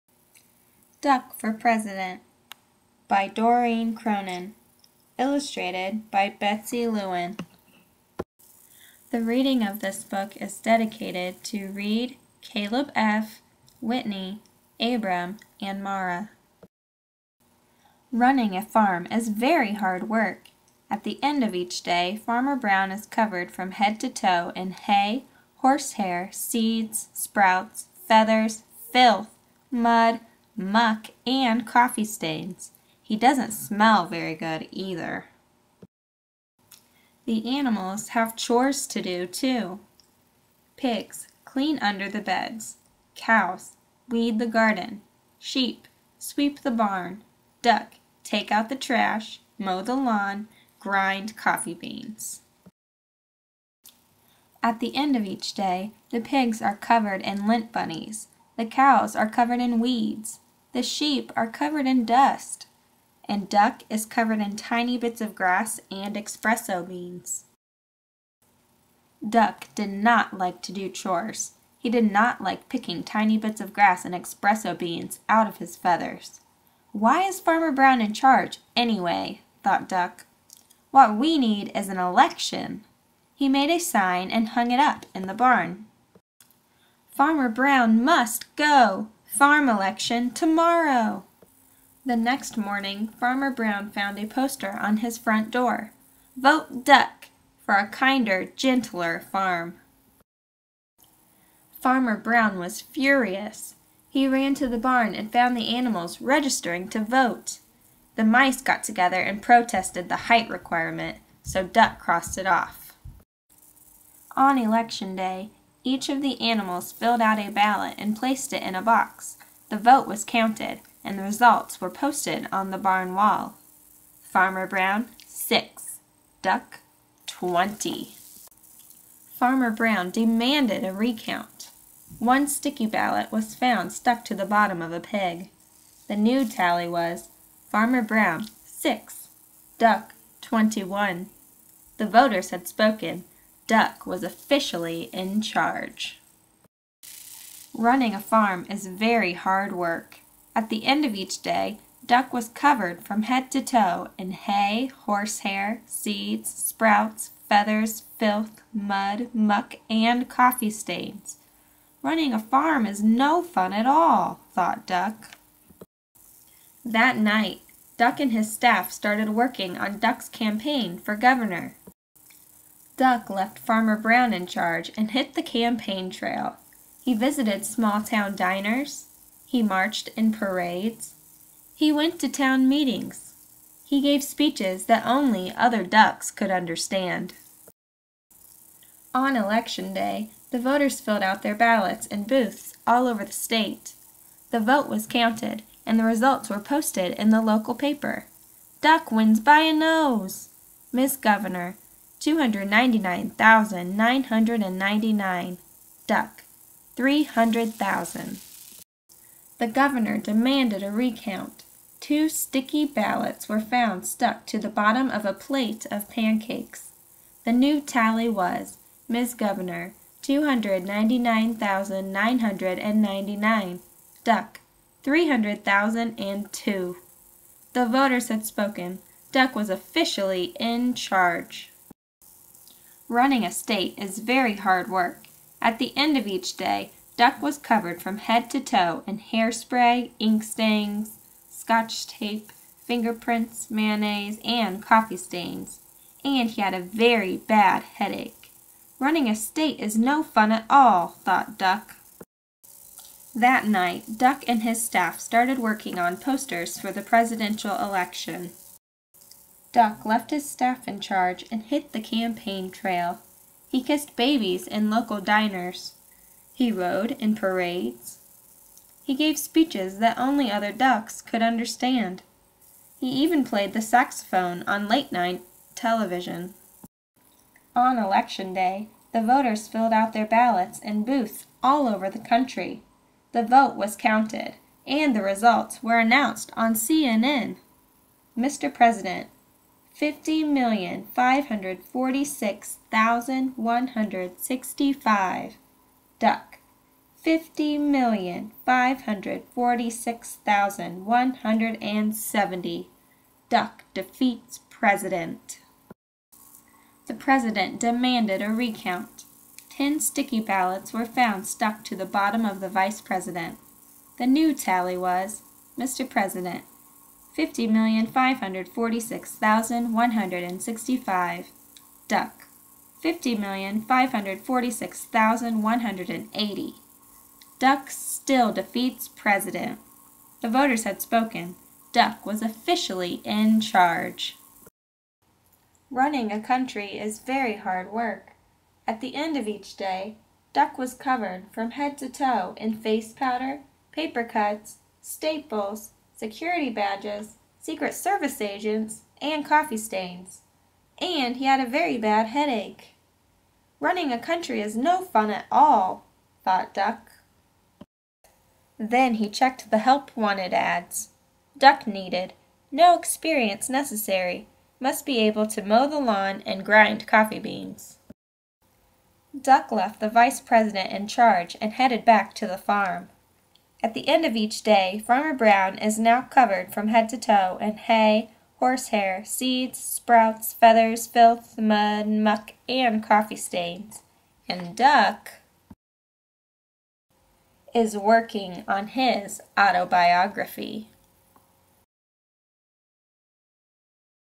Duck for President
Reading of the book "Duck for President"  by Doreen Cronin.  The reading of this book is dedicated to Reid, Kaleb F., Whitney, Abram, and Mara.